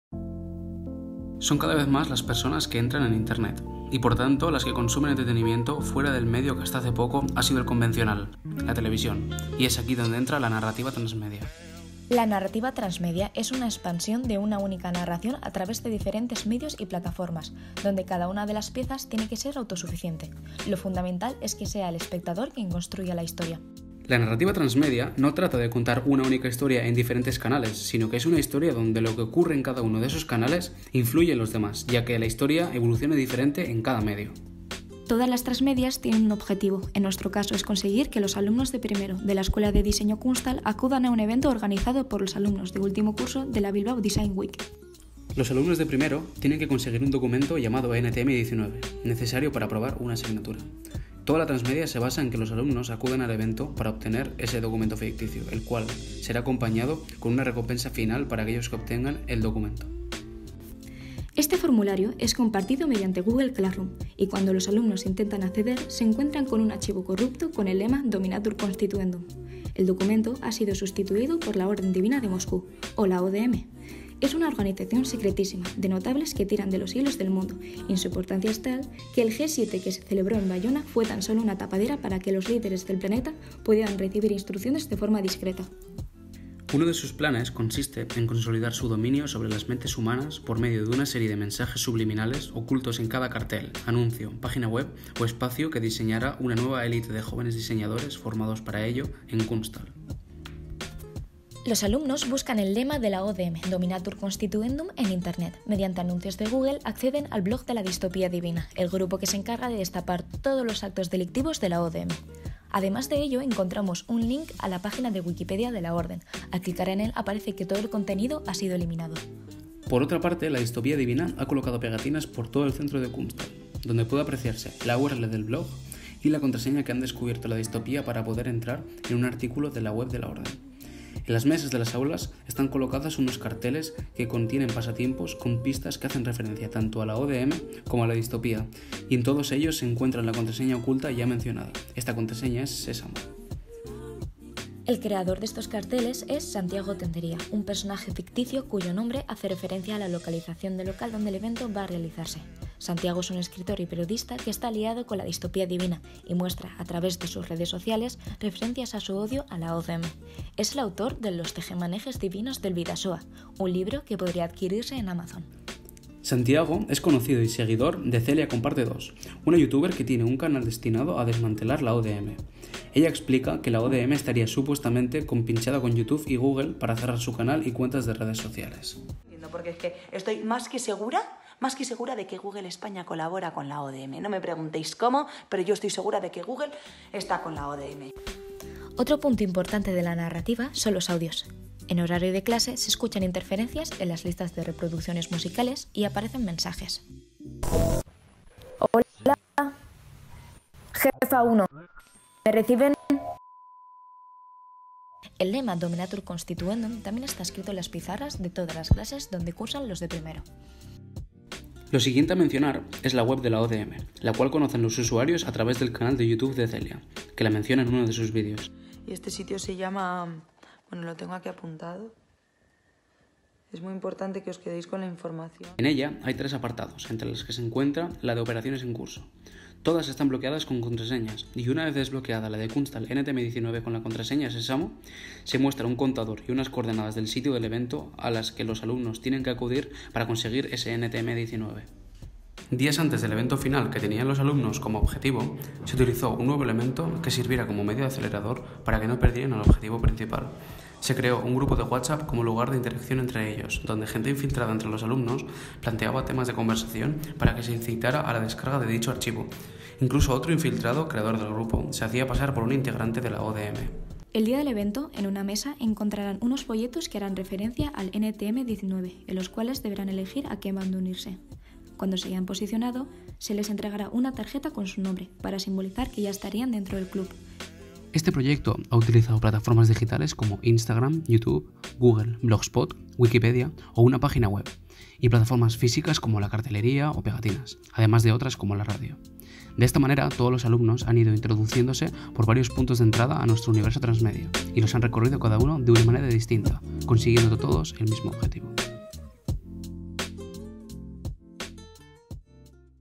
TRABAJO NARRATIVA TRANSMEDIA
Proceso de trabajo para un proyecto de la asignatura NARRATIVA TRANSMEDIA de 4º TSD Gráfico, tutorizado por Borja Legarda y explicado por sus alumnos; muchas veces nos preguntan cómo se elaboran los trabajos en nuestro Centro, desde qué diferentes puntos de vista se abordan: he aquí un ejemplo de desarrollo conceptual y materialización de un proyecto para una asignatura concreta.

Alumnado participante:
- Alex Moro.
- Cristina Terroso (voz vídeo).
- Daniel Aparicio.
- Pablo Indias (voz vídeo).
- Sara Beristain.